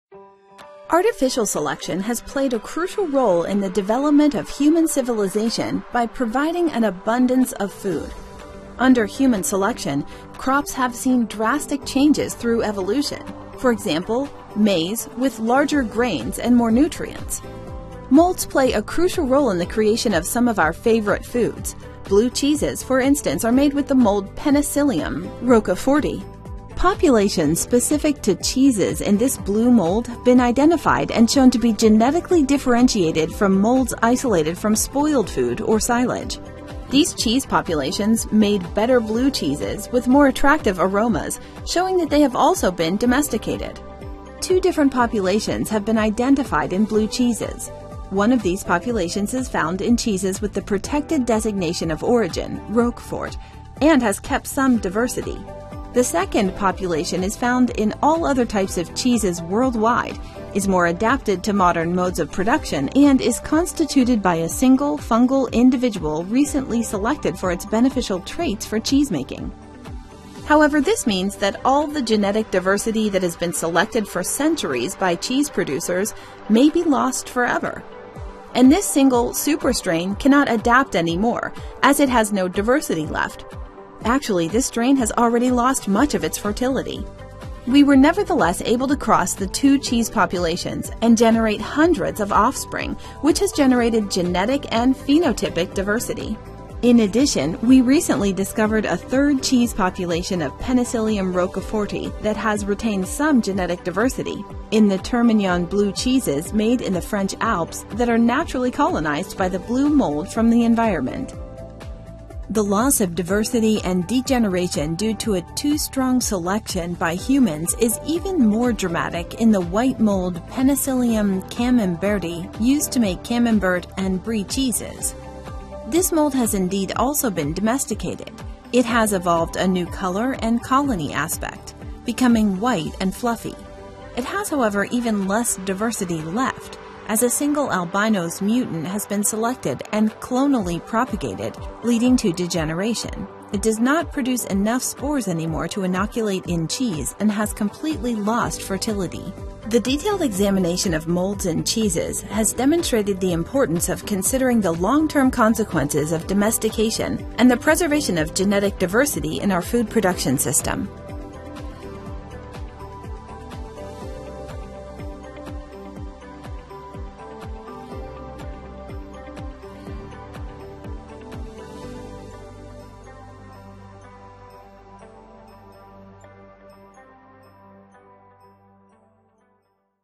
Domestication & preservation of genetic diversity in our food production system - Fungadapt project
Artificial selection has played a crucial role in the development of human civilization by providing an abundance of food. Under human selection, crops have seen drastic changes through evolution, for example, maize with larger grains and more nutrients.

Molds play a crucial role in the creation of some of our favorite foods. Blue cheeses, for instance, are made with the mold Penicillium roqueforti. Populations specific to cheeses in this blue mold have been identified and shown to be genetically differentiated from molds isolated from spoiled food or silage. These cheese populations made better blue cheeses, with more attractive aromas, showing that they have also been domesticated.

Two different populations have been identified in blue cheeses. One of these populations is found in cheeses with the protected designation of origin, Roquefort, and has kept some diversity. The second population is found in all other types of cheeses worldwide, is more adapted to modern modes of production, and is constituted by a single fungal individual recently selected for its beneficial traits for cheese making.

However, this means that all the genetic diversity that has been selected for centuries by cheese producers may be lost forever. And this single "super-strain" cannot adapt anymore, as it has no diversity left. Actually, this strain has already lost much of its fertility.

We were nevertheless able to cross the two cheese populations and generate hundreds of offspring, which has generated genetic and phenotypic diversity.

In addition, we recently discovered a third cheese population of Penicillium roqueforti, that has retained some genetic diversity, in the Termignon blue cheeses made in the French Alps, that are naturally colonized by the blue mold from the environment”.

The loss of diversity and degeneration due to a too strong selection by humans is even more dramatic in the white mold Penicillium camemberti used to make Camembert and Brie cheeses. This mold has indeed also been domesticated: it has evolved a new color and colony aspect, becoming white and fluffy. It has however even less diversity left, as a single albino’s mutant has been selected and clonally propagated, leading to degeneration. It does not produce enough spores anymore to inoculate in cheese and has completely lost fertility.

The detailed examination of molds in cheeses has demonstrated the importance of considering the long-term consequences of domestication and the preservation of genetic diversity in our food production system.